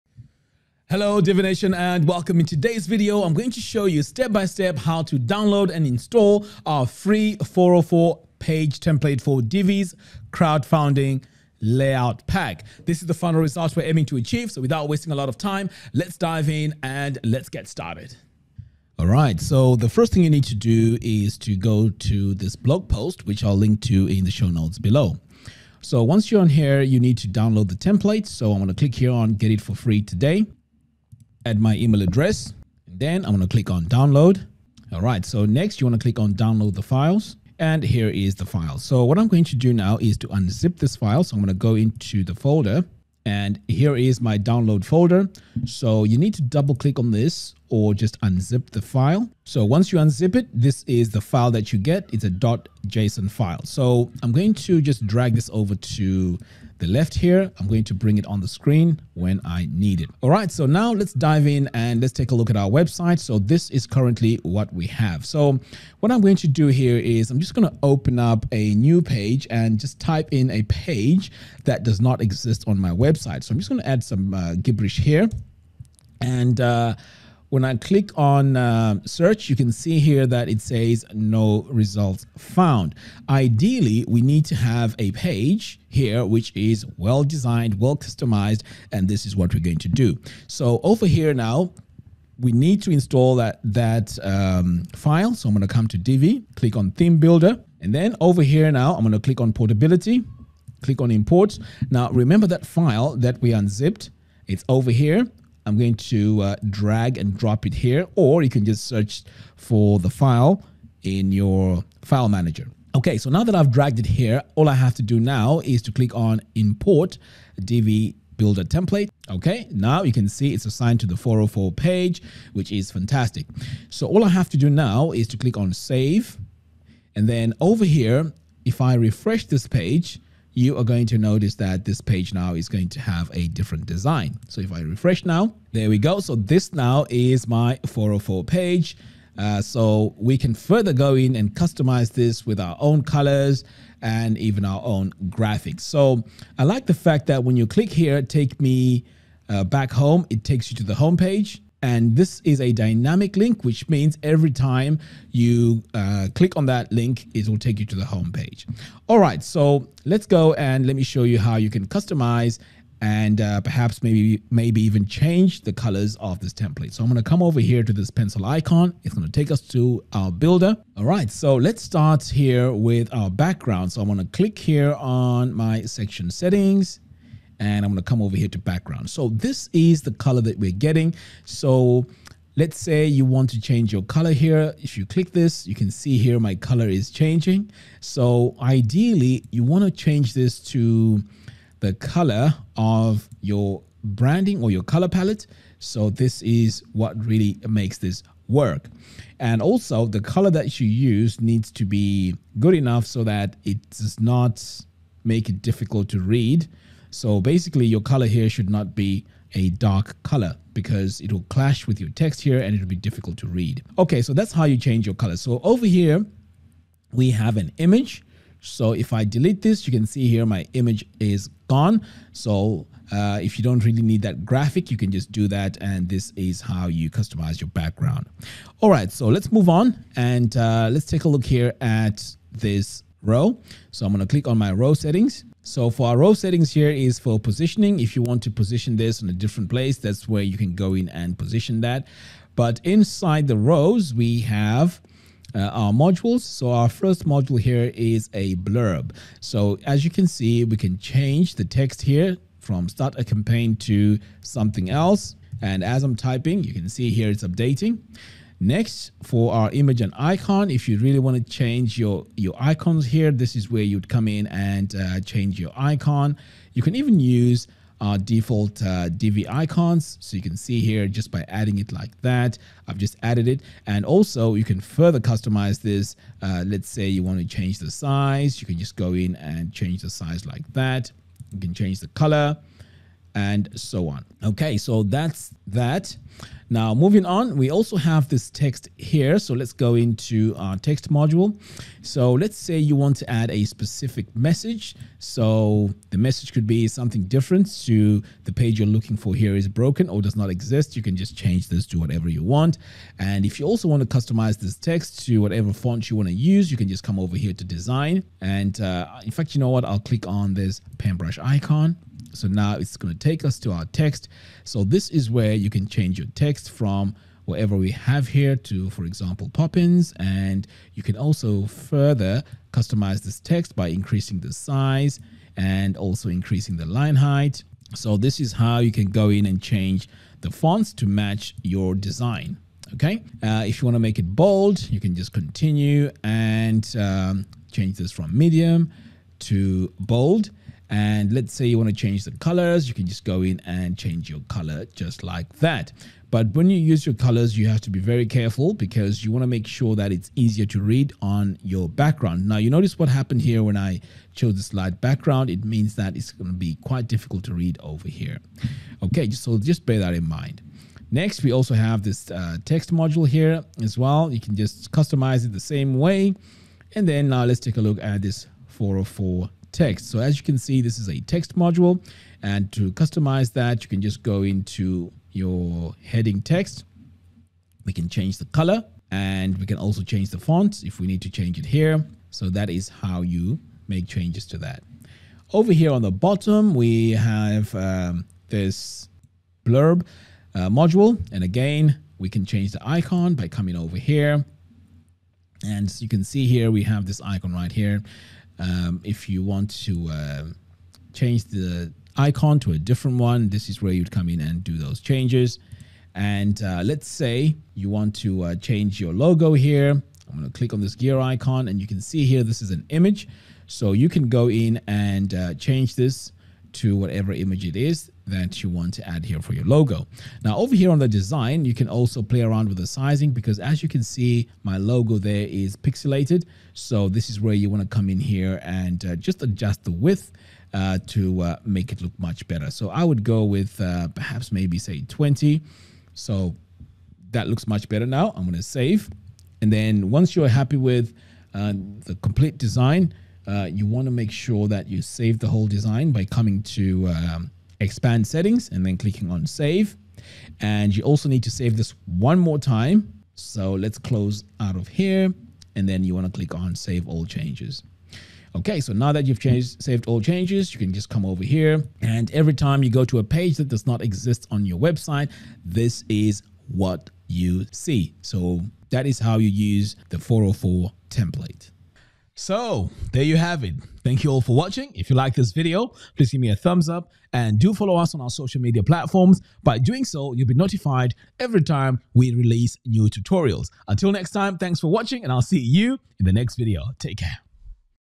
Download a FREE 404 Page Template for Divi’s Crowdfunding Layout Pack
Hey Divi Nation! Thanks for joining us for the next installment of our weekly Divi Design Initiative where each week, we give away brand new freebies. This time around we’re building upon the Crowdfunding Layout Pack with a brand new 404 page template that matches the rest of pack perfectly. Hope you enjoy it!